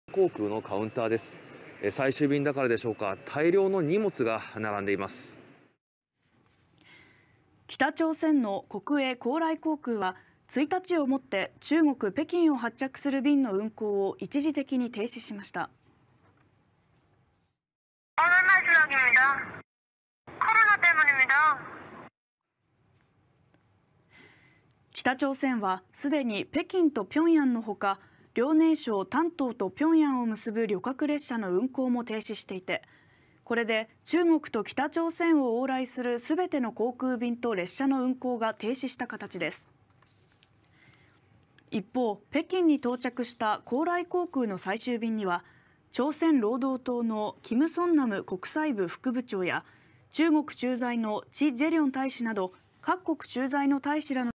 新型のコロナウイルスで？ 北朝鮮の高麗航空が中国便停止！
新型コロナウイルスの影響で、各国の航空会社が中国に発着する航空便の運航を停止する中、北朝鮮の航空会社も１日の発着便をもって、運航停止の措置を取りました。北朝鮮は既に北京と平壌のほか、遼寧省丹東と平壌を結ぶ旅客列車の運行も停止していて、これで中国と北朝鮮を往来する全ての航空便と列車の運行が停止した形です。